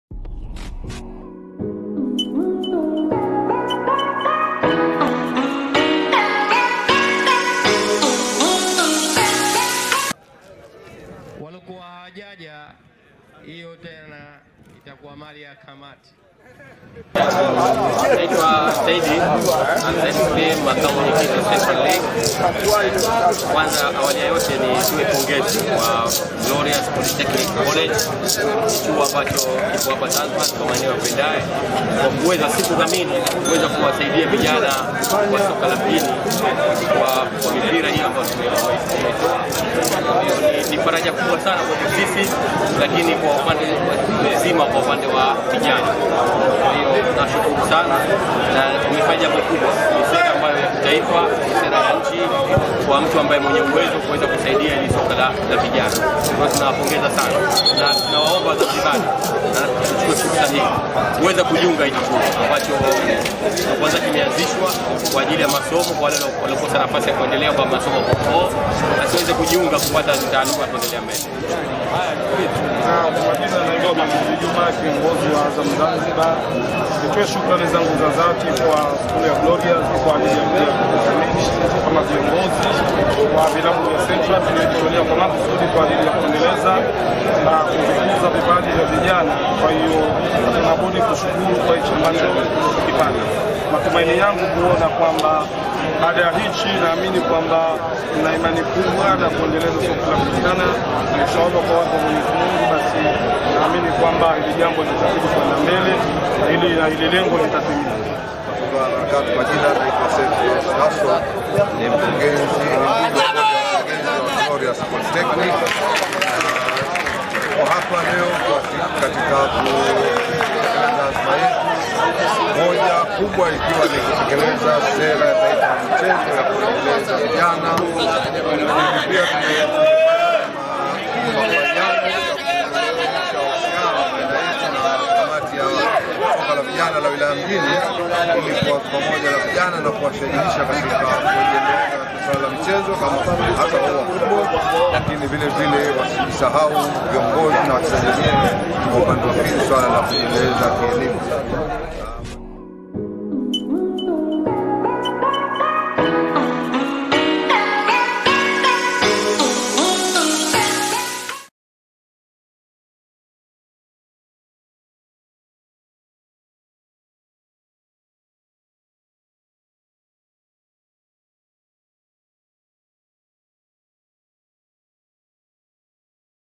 CHUO CHA GLORIOUS CHATOA MSAADA WA VIFAA VYA MICHEZO KWA TIMU ZA VIJANA|WAHAMASISHA MICHEZO NA ELIMU
Furaha ya tawala kwa timu za vijana  wapokea vifaa vya michezo kutoka katika chuo cha Glorious  waahidi kujiendeleza na masomo pamoja na michezo